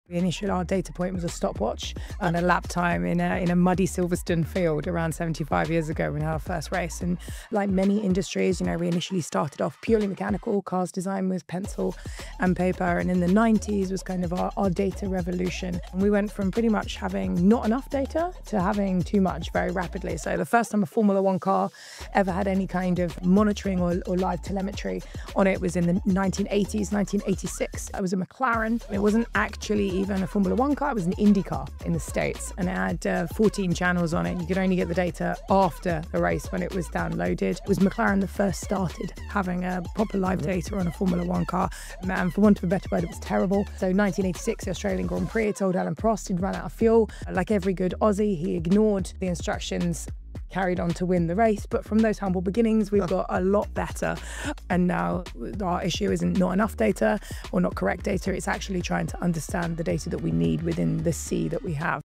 From stopwatches in muddy fields to 1.1 million data points per second
On the latest episode of Data & AI Mastery, Ruth Buscombe takes us through F1's remarkable 75-year data journey - from pencil and paper car designs to today's sensor-packed machines generating more data than most teams know what to do with.

The turning point? The 1990s data revolution flipped the script from "not enough data" to "too much data" almost overnight.

Our favourite part of this story: In 1986, McLaren's early telemetry system told Alain Prost he'd run out of fuel during the Australian Grand Prix. Like any good racer, he ignored it and won the race anyway. The data was there, but it wasn't reliable yet.

Fast forward to today, and F1 teams face the opposite challenge - filtering signal from noise in an ocean of information. It's not about collecting more data anymore. It's about identifying which data actually moves the needle.
Sound familiar? Most businesses have moved past the "we need more data" phase and straight into "which data actually matters for our goals?"

Ruth's experience shows us that the real competitive advantage isn't in having the most data - it's in knowing which questions to ask of it.

Want to hear more about how F1 teams turn data into split-second decisions at 200mph? Listen to the full conversation on the Data and AI Mastery podcast.